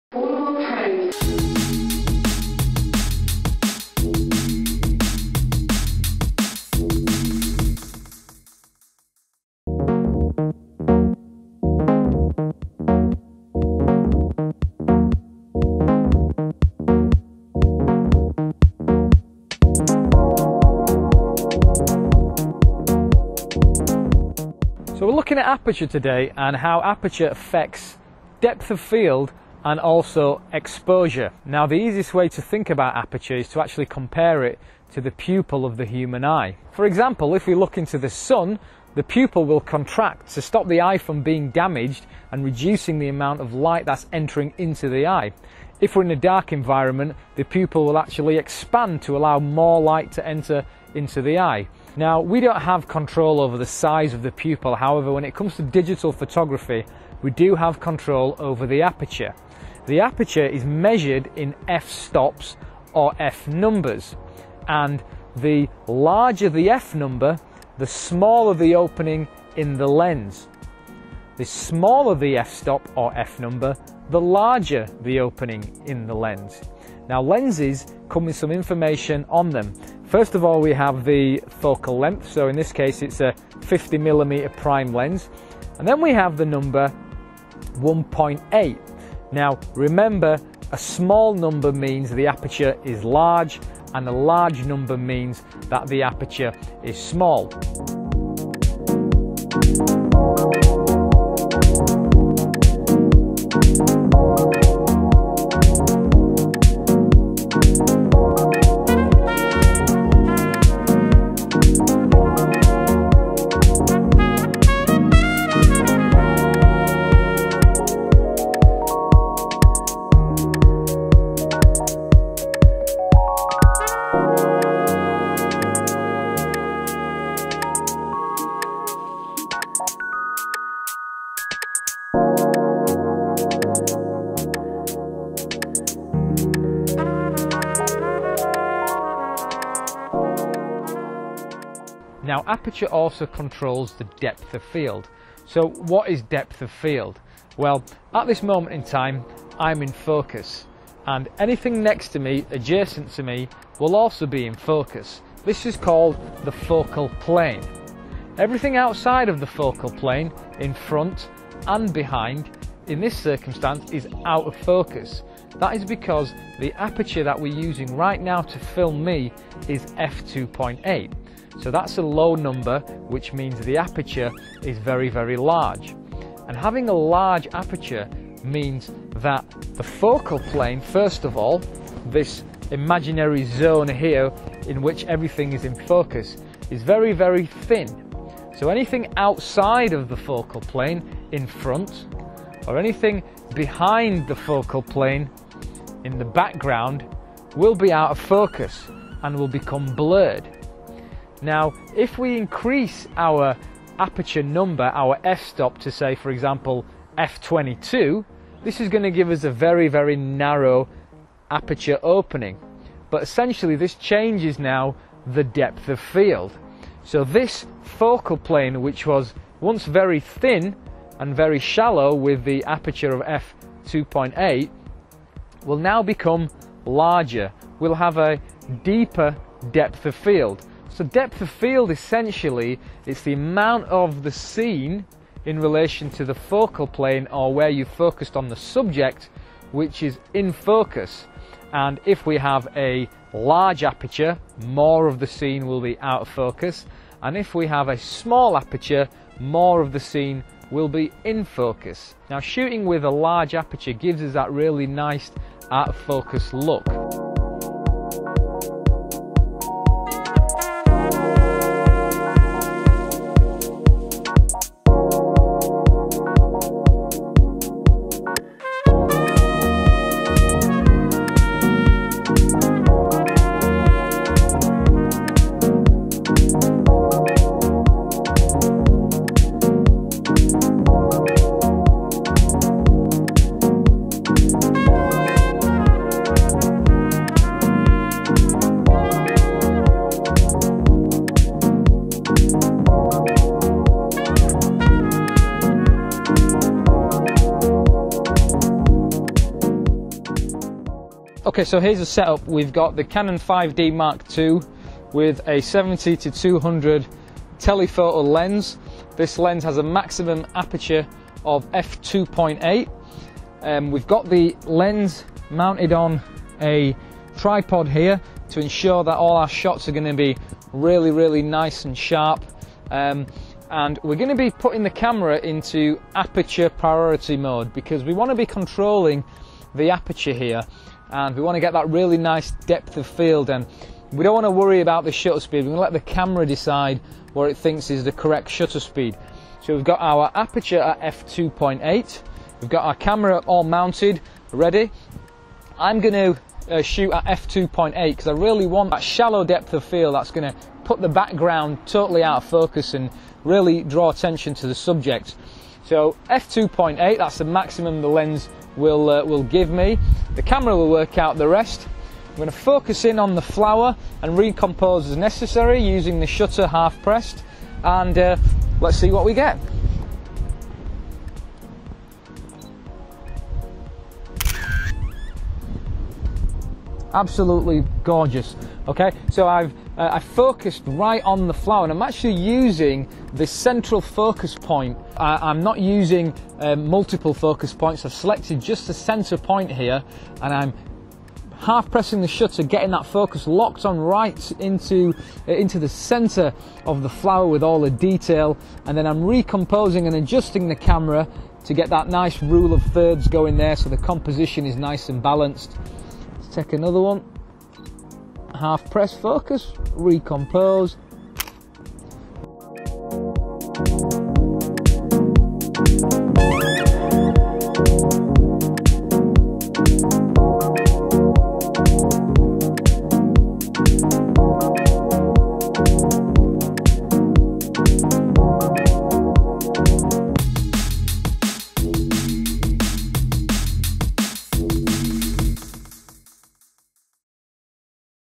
Photography fundamentals: Aperture
The aperture is an opening found inside of the camera lens and along with the shutter speed controls the amount of light allowed to reach the cameras sensor. The word aperture means opening and also controls the amount of the image outside of the focal plane that appears sharp, this is called the depth of field.

The larger the aperture opening the more light is allowed to enter the camera and the shallower/narrower/smaller the depth of field - more of the image outside the focus point is out of focus. Large aperture openings have a small f number e.g. f1.2 / f1.4 / f1.6 / f1.8 / f2.8

The smaller the aperture opening the less light is allowed to enter the camera and the deeper/larger/wider the depth of field - more of the image outside the focus point is in focus. Small aperture openings have a large f numer e.g. f8 / f11 / f16 / f22

This video aims to explain how aperture controls both the depth of field and exposure.